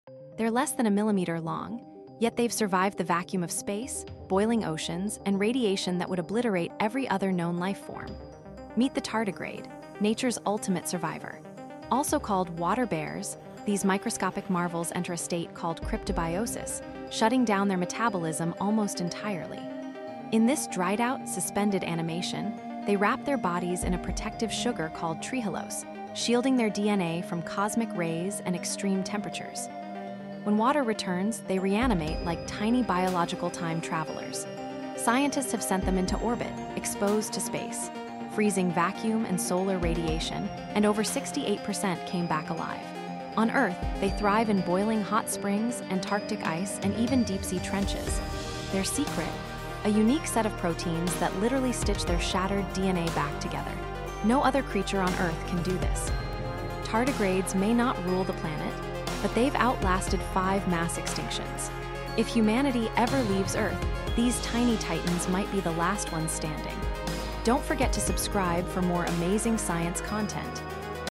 Ultimate Survivors: The Incredible Tardigrades
Discover the amazing abilities of tardigrades, nature's ultimate survivors, as they withstand extreme conditions and outlast mass extinctions. Explore their unique adaptations and potential implications for the future of life on Earth and beyond.